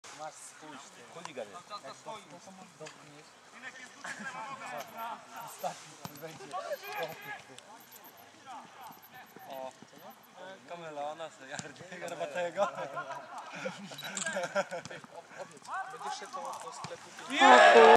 Lubomino Wilczkowo GLKS Jonkowo druga bramka meczu Wilczkowo
BRAMKA NA 2:0 Wilczkowo